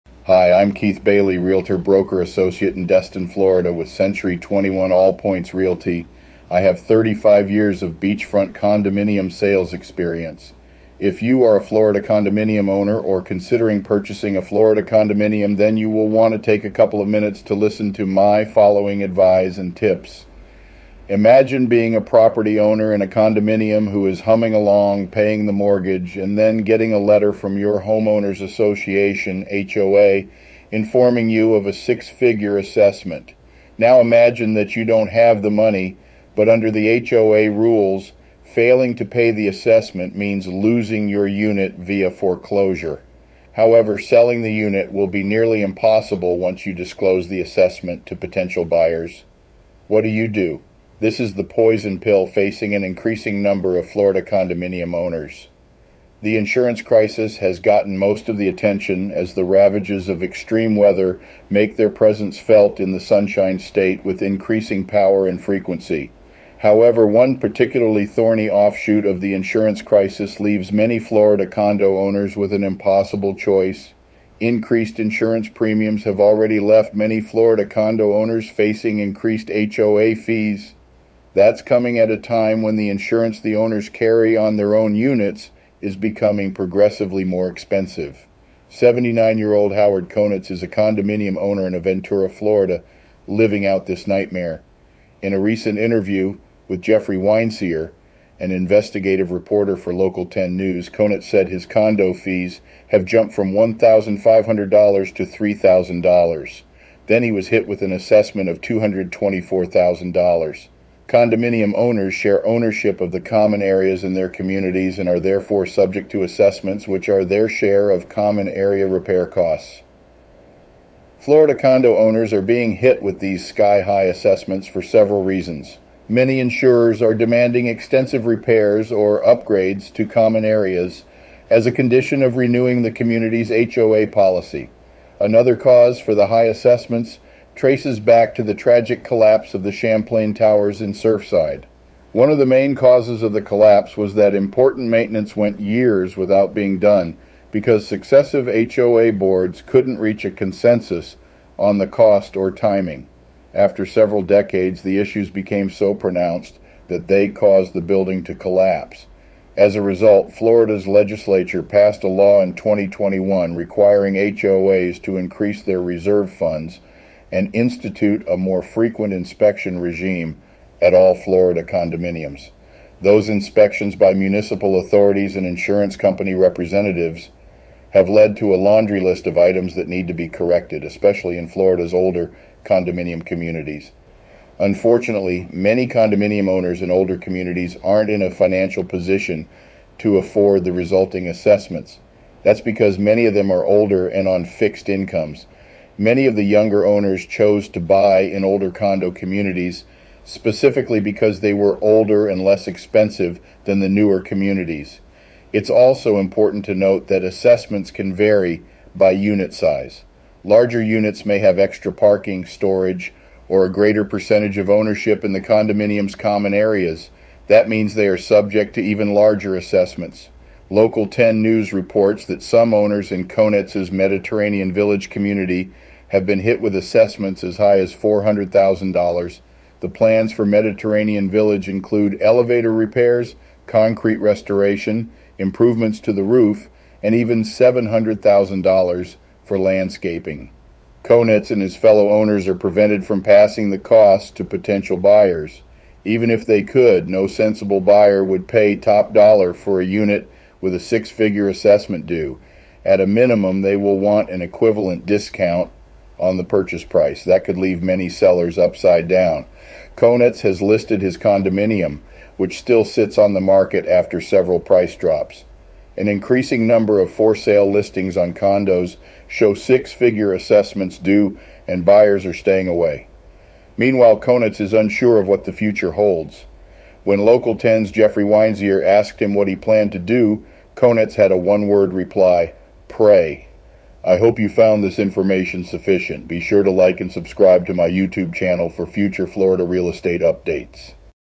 Avoid These Florida Condo Special Assessment Risks
Are you a Florida condo owner? Watch this video to learn about the dangers of special assessments in Florida condominiums. Don't get caught off guard - be prepared and informed about the risks when it comes to owning Florida real estate.

Chapters

0:00 Introduction
0:20 HOA Informs Owners of Assessment's
0:49 What do you do?
1:29 Real Owner Example
2:00 Florida condo owners are being hit with these assessments